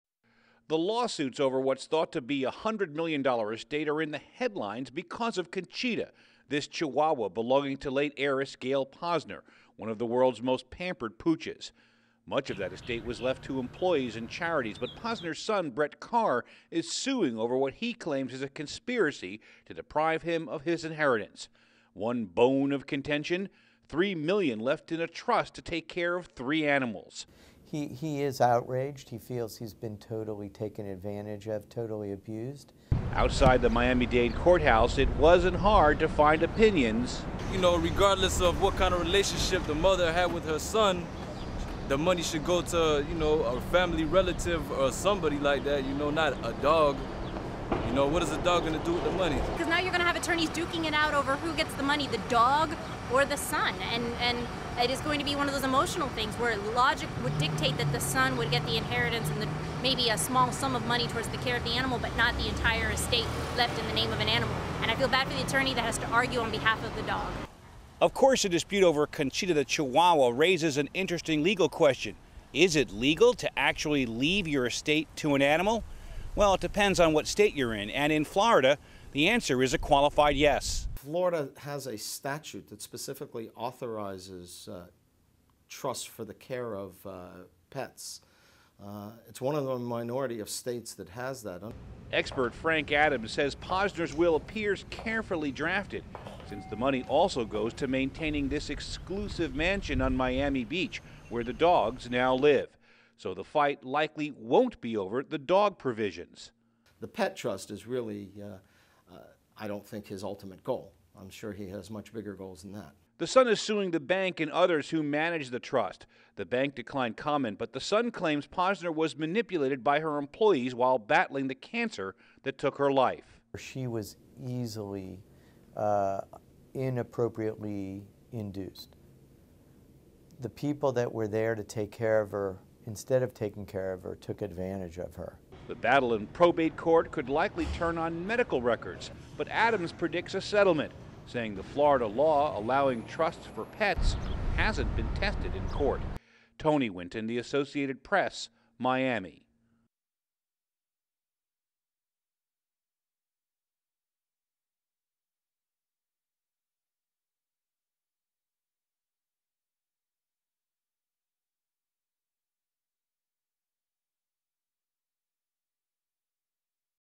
In her will, heiress Gail Posner left $3,000,000 for the care of her pet chihuahua, Chonchita, and t
(28 Jun 2010) HEADLINE: Can a Chihuahua Get $3M From Will?
NB. THIS IS A VOICEOVER TRANSCRIPT, NOT A FULL SHOTLIST
CAPTION:  In her will, heiress Gail Posner left $3,000,000 for the care of her pet chihuahua, Chonchita, and two other animals. Now, the heiress' son is claiming that the pets trust is an attempt to deprive him of his inheritance. (June 28)
THE LAWSUITS OVER WHAT'S THOUGHT TO BE A 100 MILLION ESTATE ARE IN THE HEADLINES BECAUSE OF CONCHITA -- THIS CHIHUAHUA BELONGING TO LATE HEIRESS GAIL POSNER -- ONE OF THE WORLD"S MOST PAMPERED POOCHES.
Much of that estate was left to employees and charities
but Posner's son , BRETT CARR, IS SUING OVER WHAT HE CLAIMS IS A CONSPIRACY TO DEPRIVE HIM OF HIS INHERITANCE.
ONE BONE OF CONTENTION:  THREE MILLION, LEFT IN A TRUST TO TAKE CARE OF THREE ANIMALS.
SOT BRUCE KATZEN ATTORNEY "HE IS OUTRAGED, HE FEELS HE'S BEEN TOTALLY TAKEN ADVANTAGE OF, TOTALLY ABUSED"
OUTSIDE THE MIAMI-DADE COURTHOUSE -- IT WASN'T HARD TO FIND OPINIONS
SOT CHRISTOPHER WEEKS: REGARDLESS OF WHAT KIND OF RELATIONSHIP THE MOTHER HAD WITH HER SON,THE MONEY SHOULD GO TO A FAMILY RELATIVE OR SOMEBODY LIKE THAT. YOU KNOW, NOT A DOG. WHAT IS THE DOG GOING TO DO WITH THE MONEY
SOT CLARISSA RODRIGUEZ "NOW YOU'RE GOING TO HAVE ATTORNEYS DUKING IT OUT OVER WHO GETS THE MONEY. THE DOG OR THE SON, AND ITS GOING TO BE ONE OF THOSE EMOTIONAL THINGS, WHERE LOGIC WOULD DICTATE THAT THE SON WOULD GET THE INHERITANCE AND MAYBE A SMALL SUM OF MONEY TOWARDS THE CARE OF THE ANIMAL. AND I FEEL BAD FOR THE ATTORNEY THAT HAS TO ARGUE ON BEHALF OF THE DOG.
WINTON STANDUP: of course the dispute over Conchita the Chihuahua raises an interesting legal question. Is it legal to actually leave your estate to an animal? Well, it depends on what state you're in. And in Florida, the answer is a qualified yes.
SOT FRANK ADAMS "Florida has a statute that specifically authorizes trusts for the care of pets. It's one of the minority of states that has that"
EXPERT FRANK ADAMS SAYS POSNER'S WILL APPEARS CAREFULLY DRAFTED, SINCE THE MONEY ALSO GOES TO MAINTAINING  THIS EXCLUSIVE MANSION ON MIAMI BEACH WHERE THE DOGS NOW LIVE. SO THE FIGHT LIKELY WON'T BE OVER THE DOG PROVISIONS
SOT FRANK ADAMS, PROBATE LAWYER "The pet trust, is really, I don't think, his ultimate goal. I'm sure he has much bigger goals than that."
THE SON IS SUING THE BANK AND OTHERS WHO MANAGED THE TRUST. THE BANK DECLINED COMMENT, BUT THE SON CLAIMS POSNER WAS
WAS MANIPULATED BY HER EMPLOYEES WHILE BATTLING THE CANCER THAT TOOK HER LIFE
'SOT BRUCE KATZEN ATTORNEY "IT LEFT HER IN A SITUATION WHERE SHE WAS EASILY, INAPPROPRIATELY INDUCED. THE PEOPLE THAT WERE THERE TO TAKE CARE OF HER -- INSTEAD OF TAKING CARE OF HER -- TOOK ADVANTAGE OF HER.
THE BATTLE IN PROBATE COURT COULD LIKELY TURN ON MEDICAL RECORDS - BUT ADAMS PREDICTS A SETTLEMENT -- SAYING THE FLORIDA LAW ALLOWING TRUSTS FOR PETS HASN"T BEEN TESTED IN COURT.
TONY WINTON THE ASSOCIATED PRESS - MIAMI